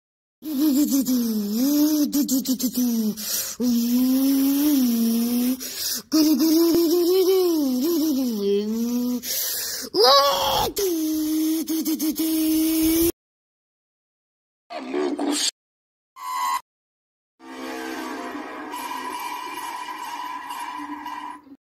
Europe goes brooom but gets caught by AMOGUS
Hello there.
Yay. thank you for the support and thanks to NovaScotia to give me the last few subs to get to 100. Also big thanks to State Hollandia Mapping for giving me a shoutout and helping me make a channel and grow it.
So I decided to make a fun video.
I hope you enjoyed it.
Byee!